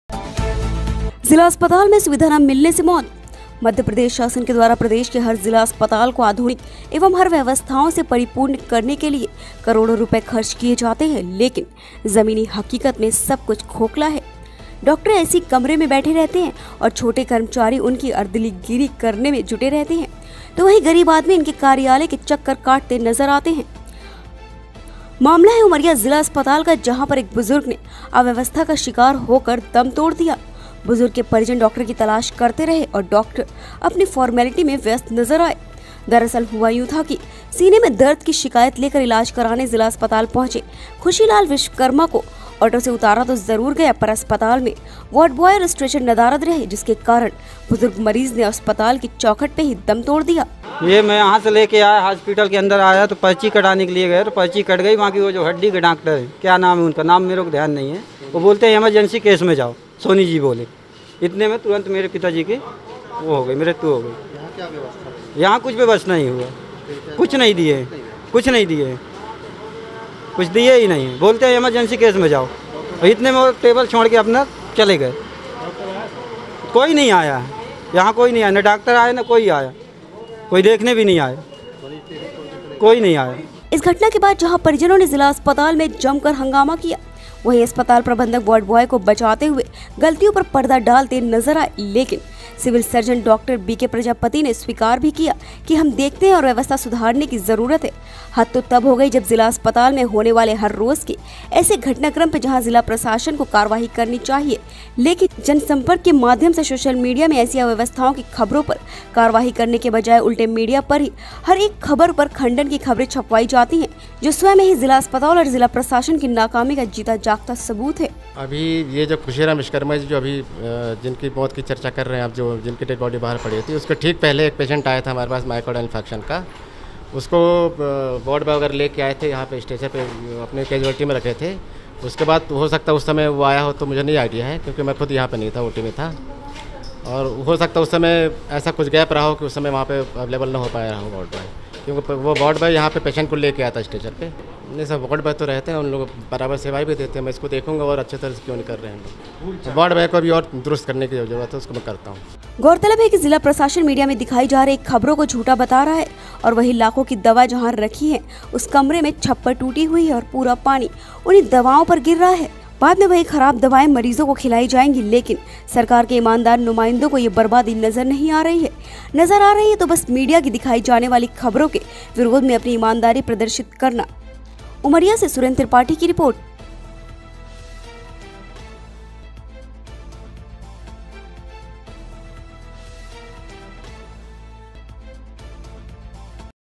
उमरिया:-जिला अस्पताल में सुविधा न मिलने से मौत
म. प्र. शासन के द्वारा प्रदेश के हर जिला अस्पताल को आधुनिक एवं हर व्यवस्थाओं से परिपूर्ण करने के लिए करोड़ो रुपये खर्च किये जाते है, लेकिन जमीनी हकीकत में सब कुछ खोखला है, डाक्टर एसी कमरों में बैठे रहते हैं और छोटे कर्मचारी उनकी अर्दलीगिरी करने मे जुटे रहते है तो वहीं गरीब आदमी इनके कार्यालयों के चक्कर काटते नजर आते हैं।

मामला है उमरिया जिला अस्पताल का है जहां पर एक बुजुर्ग अव्यवस्था का शिकार होकर दम तोड़ दिया, बुजुर्ग के परिजन डाक्टर की तलाश करते रहे और डाक्टर अपनी फारमल्टी में व्यस्त नजर आये। दरअसल हुआ यूं था कि सीने में दर्द की शिकायत लेकर इलाज कराने जिला अस्पताल पहुंचे खुशी लाल विश्वकर्मा को आटो से उतारा तो जरुर गया पर अस्पताल में वार्डबॉय और स्ट्रेचर नदारद रहे, जिसके कारण बुजुर्ग मरीज ने अस्पताल की चौखट में ही दम तोड़ दिया।

 इस घटना के बाद जहां परिजनों ने जिला अस्पताल में जमकर हंगामा किया वहीं अस्पताल प्रबंधन वार्डबॉयों को बचाते हुए गलतियों पर पर्दा डालते तो नजर आया लेकिन सिविल सर्जन डॉक्टर बी के प्रजापति स्वीकार भी किये कि हम देखते हैं और व्यवस्था सुधारने की जरूरत है। हद तो तब हो गई जब जिला अस्पताल में होने वाले हर रोज के ऐसे घटनाक्रम पर जहां जिला प्रशासन को कार्यवाही करनी चाहिए लेकिन जनसंपर्क के माध्यम से सोशल मीडिया में ऐसी अव्यवस्थाओं की खबरों पर कार्यवाही करने की बजाय उल्टे मीडिया की हर एक खबर पर खंडन की खबरें छपवाई जाती है, जो स्वमेव ही जिला अस्पताल और जिला प्रशासन की नाकामी का जीता जागता सबूत है।

गौरतलब है कि जिला प्रशासन मीडिया में दिखाई जा रही खबरों को झूठा बता रहा है तो वहीं लाखों की दवा जहां रखी है उस कमरे की छप्पर टूटी हुई है और पूरा पानी उन्ही दवाओं पर गिर रहा है, बाद में वही खराब दवाएं मरीजों को खिलाई जाएंगी, लेकिन सरकार के ईमानदार नुमाइंदों को यह बरबादी नजर नही आ रही है। नजर आ रही है बस मीडिया में दिखाई जाने वाली खबरों के विरोध में अपनी ईमानदारी प्रदर्शित करना।